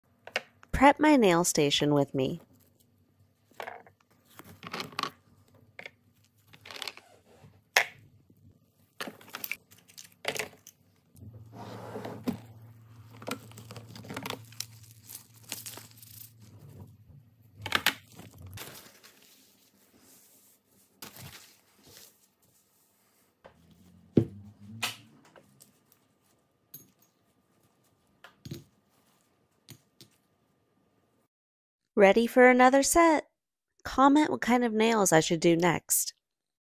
ASMR | set up my nail station 💅🏽
Listen and watch me set up my nail station.  ASMR selection of chrome powders, nail tips, gel polish, and nail brushes.  Craft storage and birthday nails inspiration.  Supplies for press on nails!